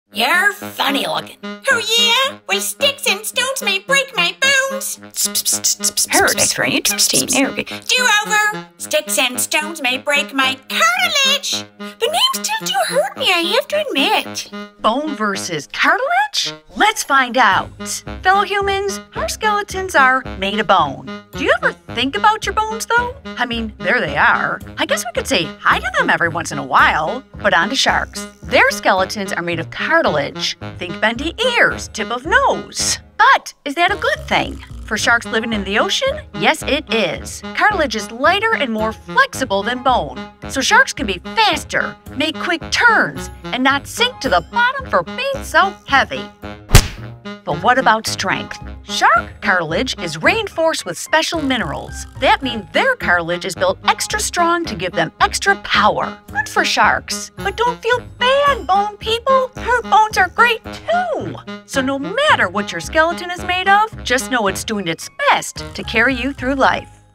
Shark vs Human?
Is shark cartilage strong like human bone? Let's find out!